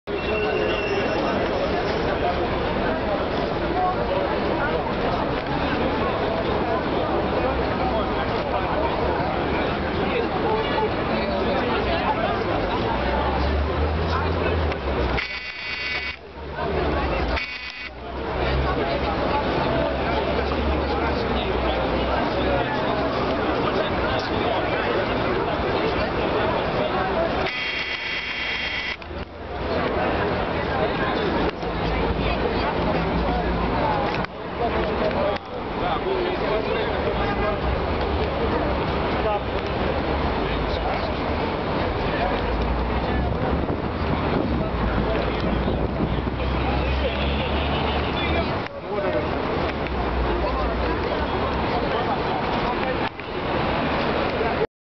Гостиный двор 31.05.2011
Перед началом митинга в защиту 31 статьи конституции РФ.